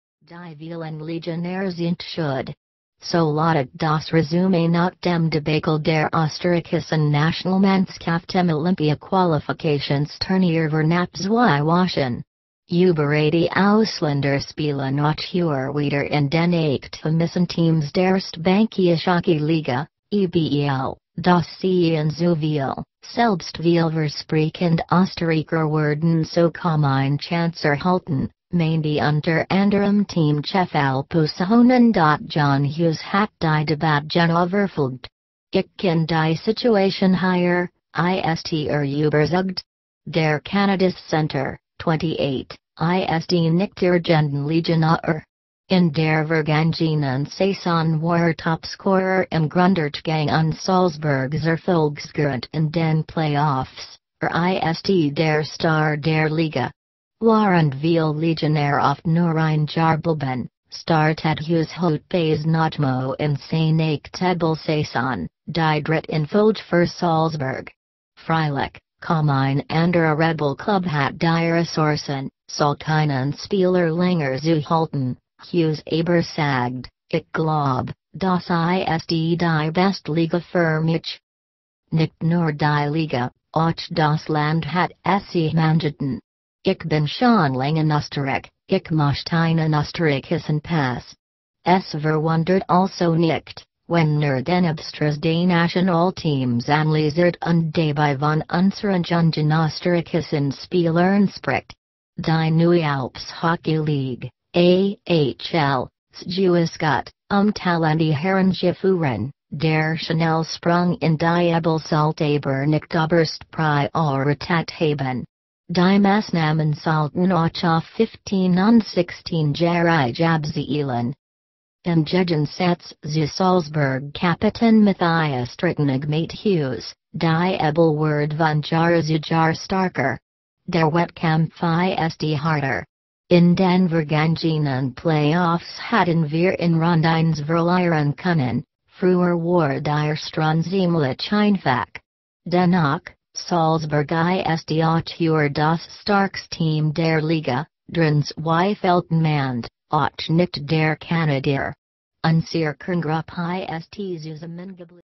Austrian Hockey League‬, ‪hockey‬‬
Die vielen Legionäre sind schuld. So lautete das Resümee nach dem Debakel der österreichischen Nationalmannschaft im Olympia-Qualifikationsturnier vor knapp zwei Wochen. Über 80 Ausländer spielen auch heuer wieder in den acht heimischen Teams der Erste Bank Eishockey Liga (EBEL), das seien zu viele, selbst vielversprechende Österreicher würden so kaum eine Chance erhalten, meinte unter anderem Teamchef Alpo Suhonen.John Hughes hat die Debatte genau verfolgt. „Ich kenne die Situation hier“, ist er überzeugt. Der kanadische Center, 28, ist nicht irgendein Legionär. In der vergangenen Saison war er Topscorer im Grunddurchgang und Salzburgs Erfolgsgarant in den Play-offs, er ist der Star der Liga. Während viele Legionäre oft nur ein Jahr bleiben, startet Hughes heute bei Znojmo in seine achte EBEL-Saison, die dritte in Folge für Salzburg. Freilich, kaum ein anderer EBEL-Klub hat die Ressourcen, solch einen Spieler länger zu halten, Hughes aber sagt: „Ich glaube, das ist die beste Liga für mich.“

Nicht nur die Liga, auch das Land hat es ihm angetan. „Ich bin schon lange in Österreich, ich möchte einen österreichischen Pass.“ Es verwundert also nicht, wenn er den Absturz des Nationalteams analysiert und dabei von „unseren jungen österreichischen Spielern“ spricht. Die neue Alps Hockey League (AHL) sei gewiss gut, um Talente heranzuführen, der schnelle Sprung in die EBEL sollte aber nicht oberste Priorität haben. Die Maßnahmen sollten auch auf 15- und 16-Jährige abzielen.

Im Gegensatz zu Salzburg-Kapitän Matthias Trattnig meint Hughes, die EBEL werde von Jahr zu Jahr stärker. „Der Wettkampf ist härter. In den vergangenen Play-offs hätten wir in Runde eins verlieren können, früher war die erste Runde ziemlich einfach.“ Dennoch: Salzburg ist auch heuer das stärkste Team der Liga, daran zweifelt niemand, auch nicht der Kanadier. „Unsere Kerngruppe ist zusammengeblieben.“ Er selbst hat noch einen Vertrag für diese Saison, möchte auch danach in Salzburg bleiben. Nicht als Legionär, sondern als Österreicher.